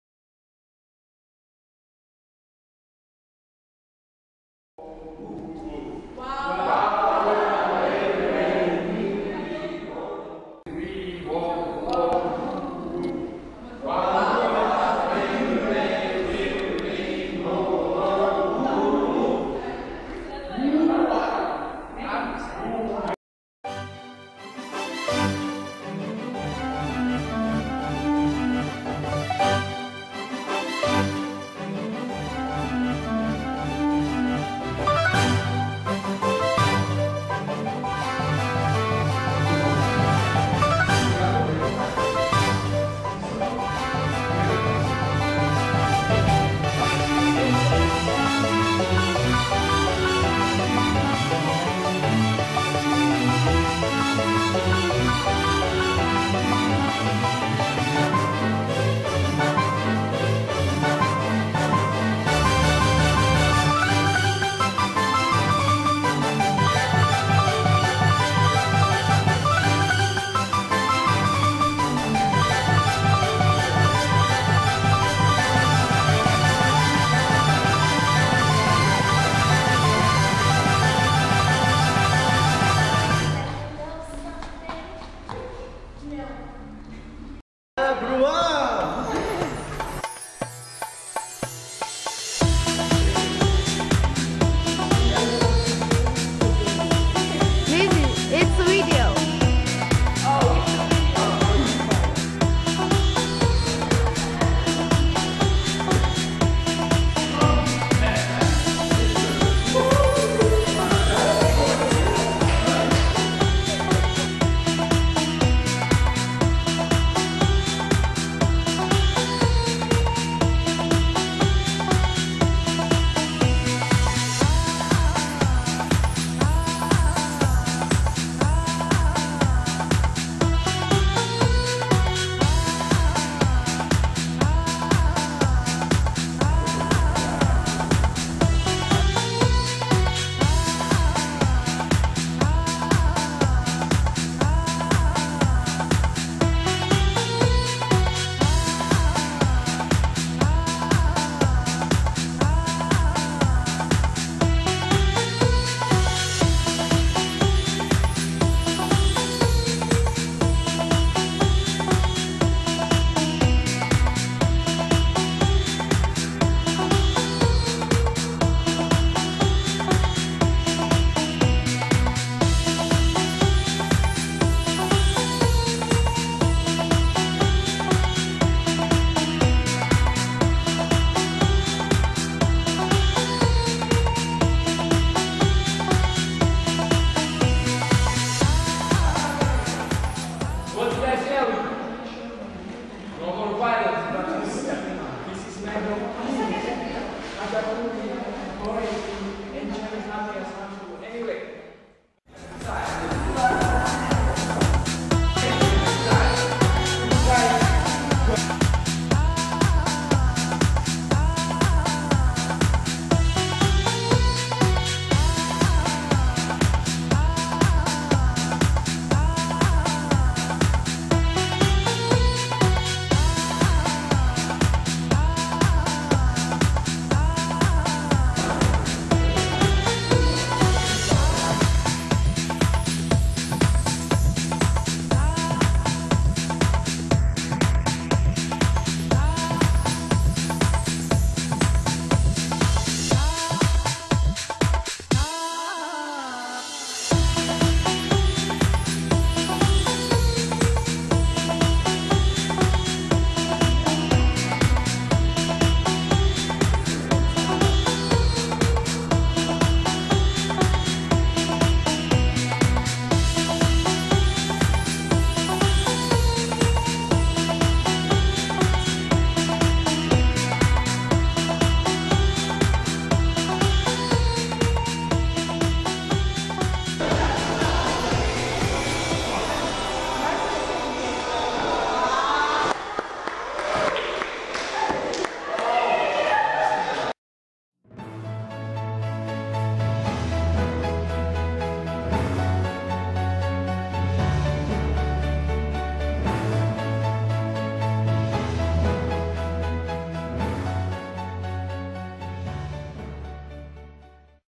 The Making of Murtazi and Juliet
Georgian version of Romeo and Juliet, Batumi, Georgia. 2012